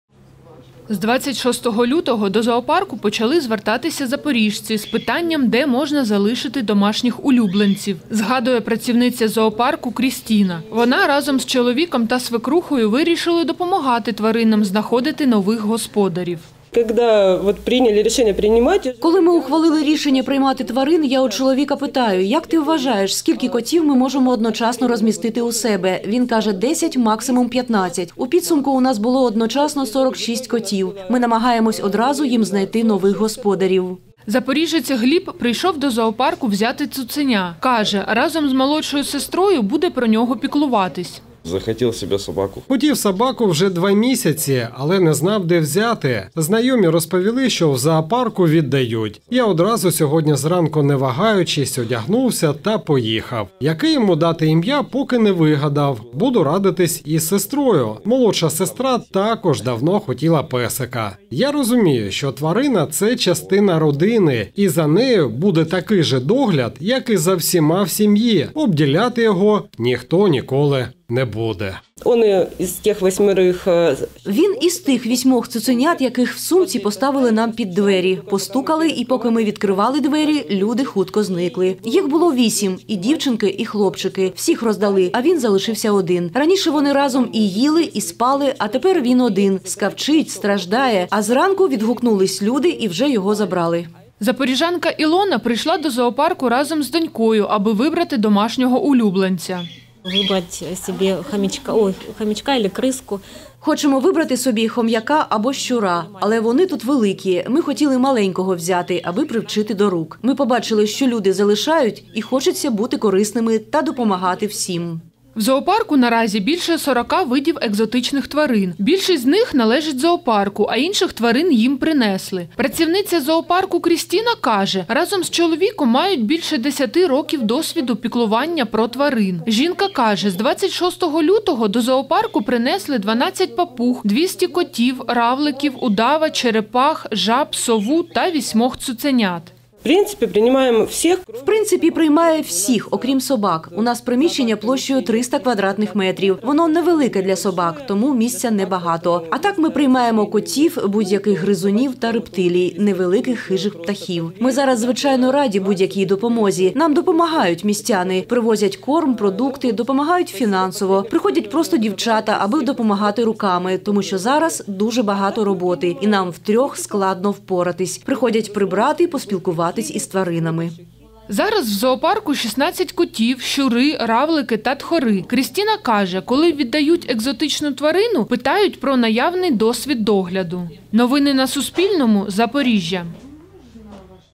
У Запоріжжі після початку війни екзотичний зоопарк почав приймати хатніх тварин | Новини
Зоопарк екзотичних тварин у центрі Запоріжжя після початку російсько-української війни приймає тварин, господарі яких вимушені були покинути місто.  Водночас Запорожці, які давно хотіли домашнього улюбленця - можуть взяти тварину до себе безплатно.